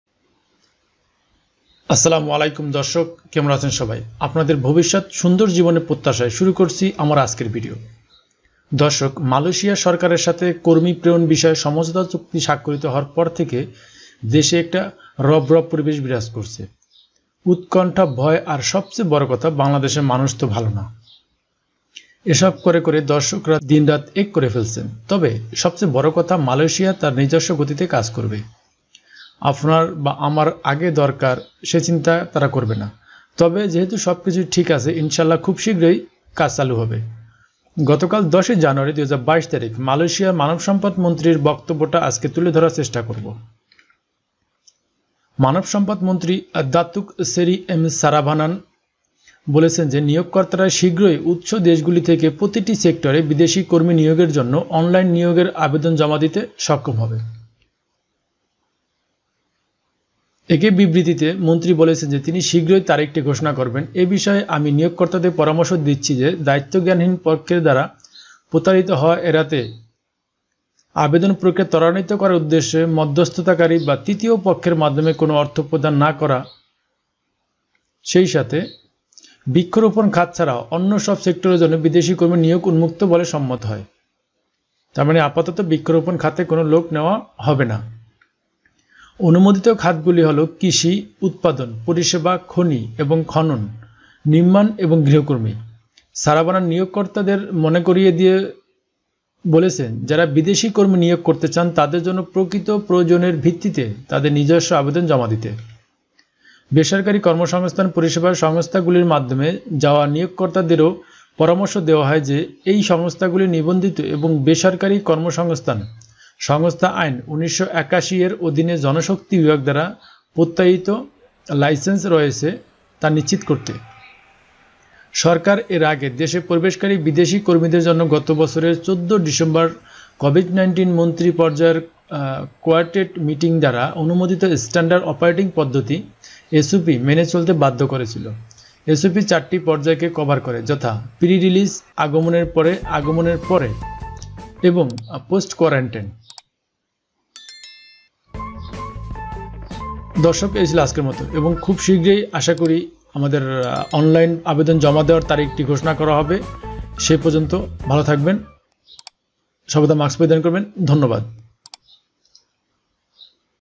কলিং ভিসার সু-সংবাদ দিলেন মানবসম্পদমন্ত্রী ll কলিং ভিসা কবে চালু হবে ll Malaysia Calling Visa ll
Helpline BD24,